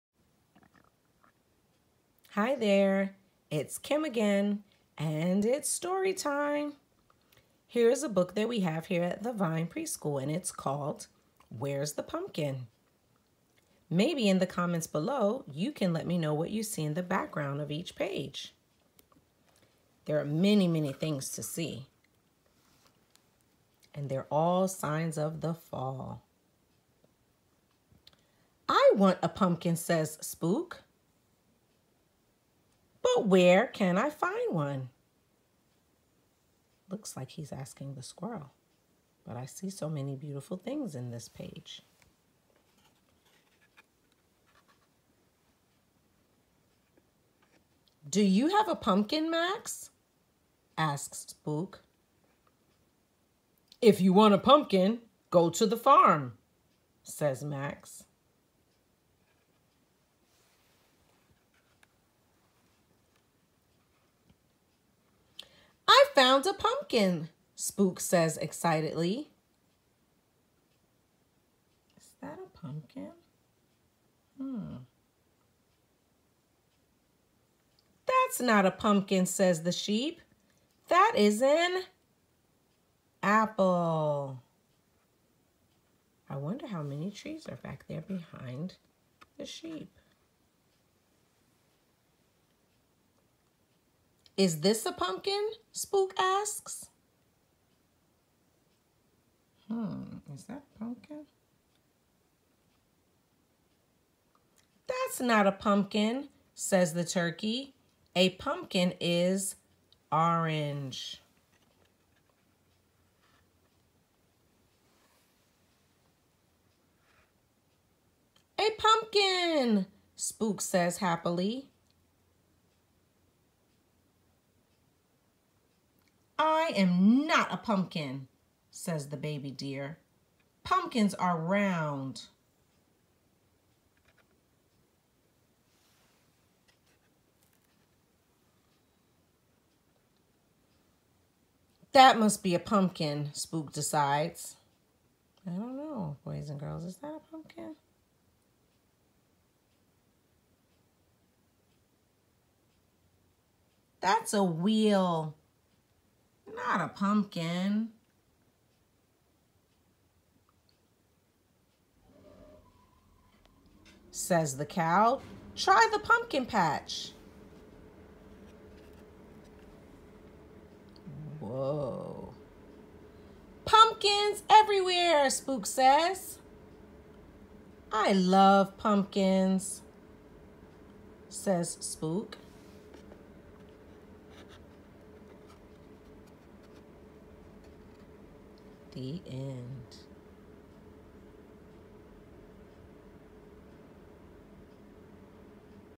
Where’s the pumpkin? by Fhiona Galloway (Illustrator) read aloud
Spook, the black cat, has heard about pumpkins, but she isn’t sure what a pumpkin is. She sets out across the farmyard to try to find out. Each animal she meets tells her something different about pumpkins. "A pumpkin is orange," says the dog, so Spook finds an orange hen. "I’m not a pumpkin!" clucks the hen. "A pumpkin is round." Will Spook ever find out what a pumpkin really is?

by Fhiona Galloway (Illustrator)